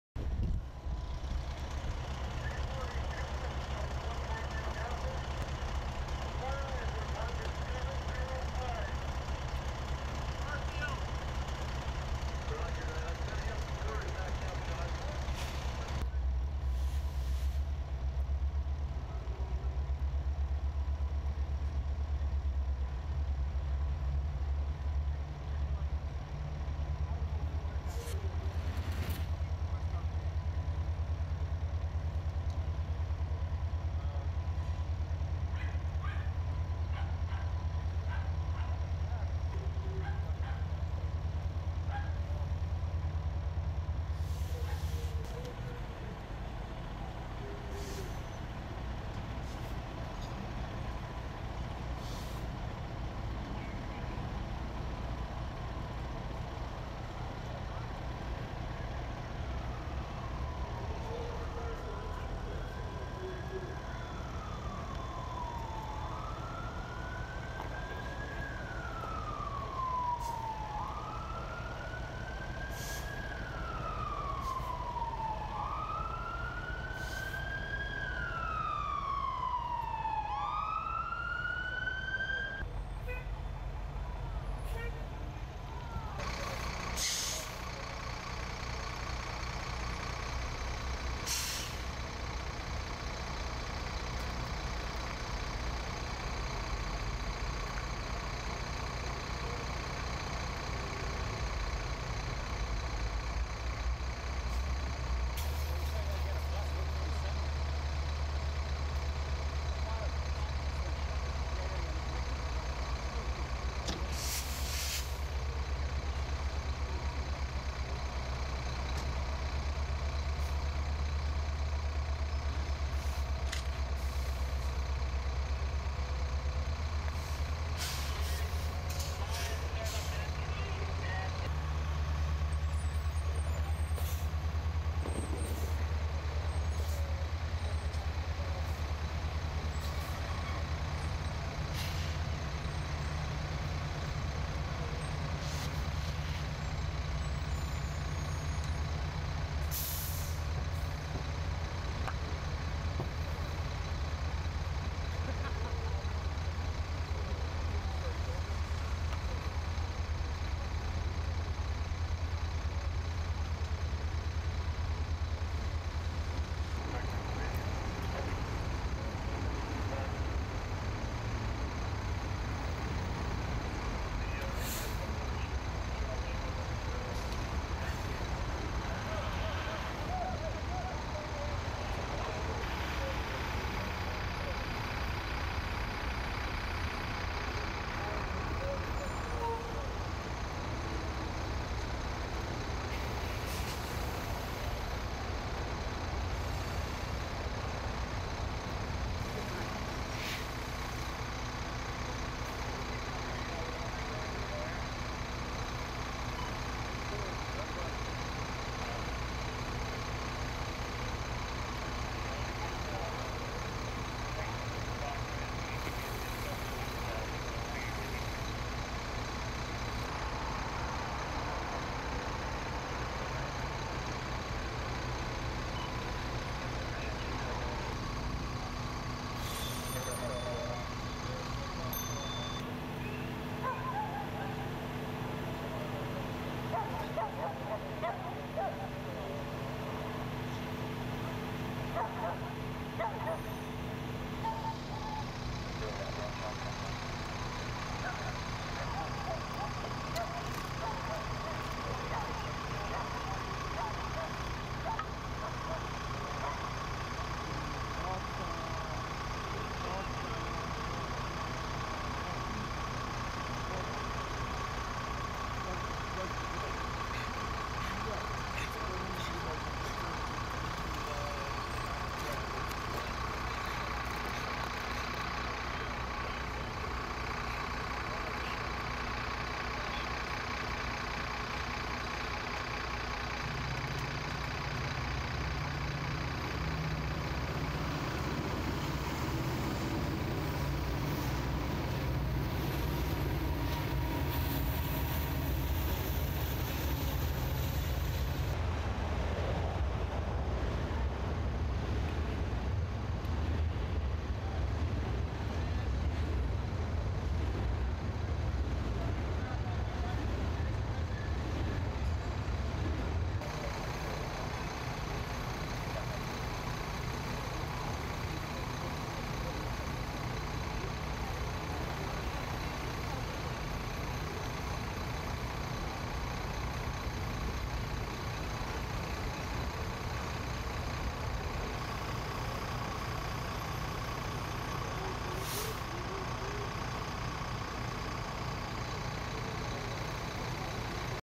3725 Dundas West Toronto
2nd Alarm Apartment Fire. Flames visible on arrival.